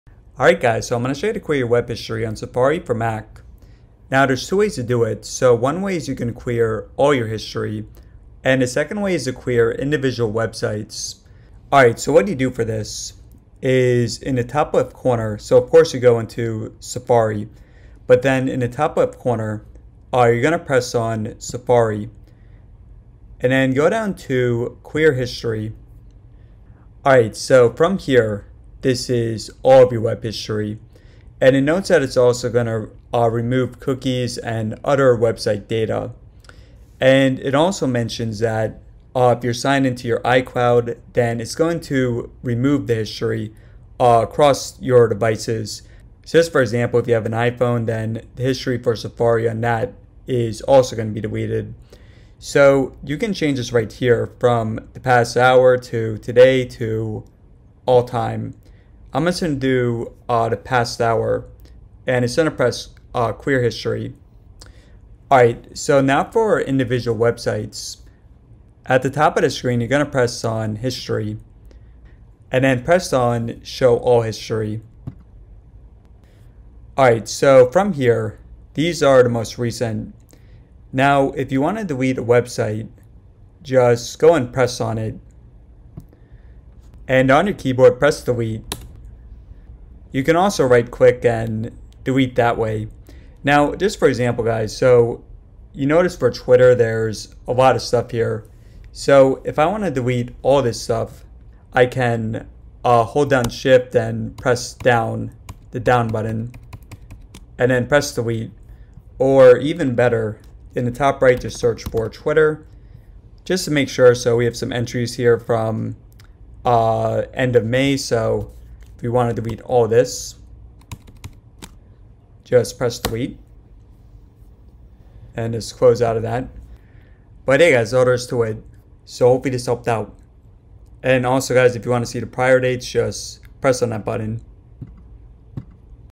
MacBook: How to Delete Safari History! (Quick)
How to delete all Safari (Mac) history or some by site or date. MacBook Pro 16 used.